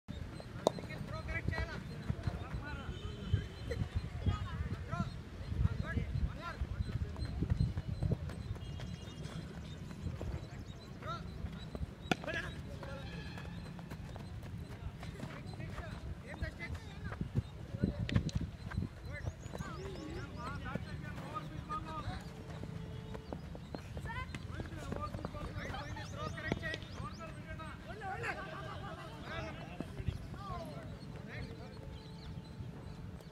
Fielding practice Nov 12, 2019
Fielding practice Nov 12, 2019. You will find Nikhil in action in this video